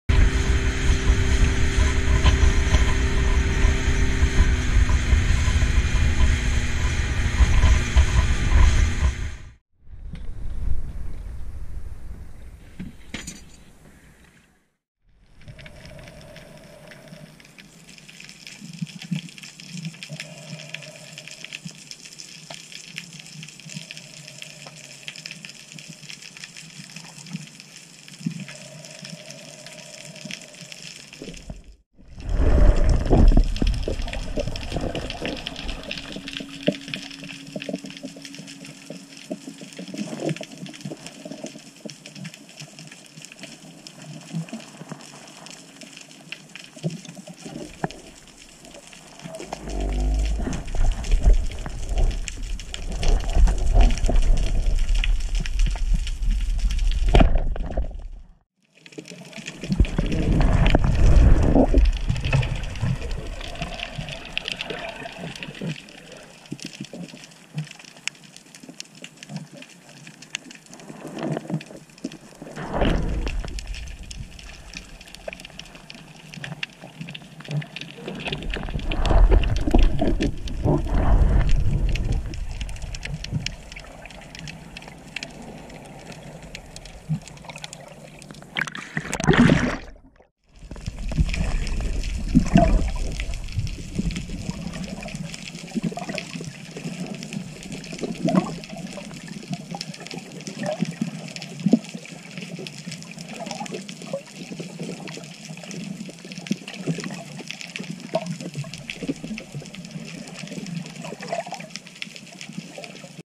Christmas Eve trip to Noises
My observation is the number of adult snapper I see is inversely proportional to kina infestation. There was quite a bit of snapper, most of them undersized, but they were not tiny juveniles hanging around a nursery. Most of the kina I saw was neatly tucked away and no obvious kina barrens. It was very contrasting compared to the Auckland East Coast and even the rocks around Rakino Island. My guess is this place is less accessible and gets less hammering.